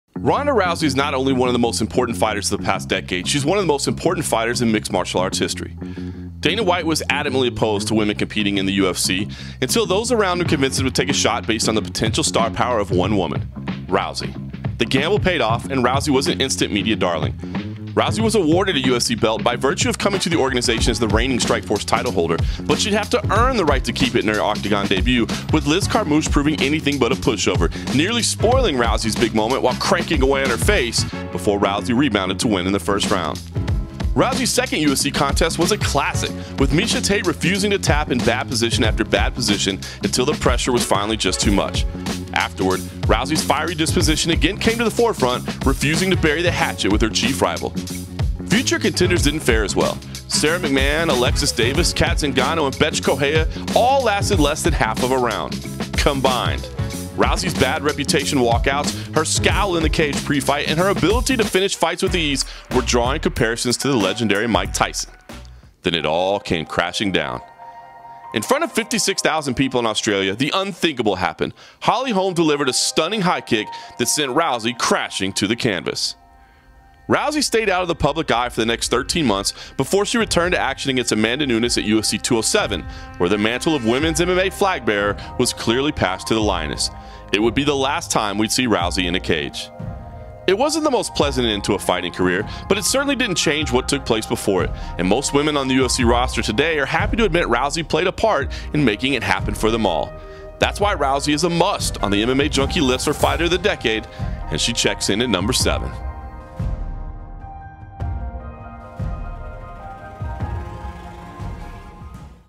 Top MMA fighters of the decade, 2010-2019: Ronda Rousey
MMA Junkie's John Morgan reflects on our No.7 fighter of the 2010s, Ronda Rousey.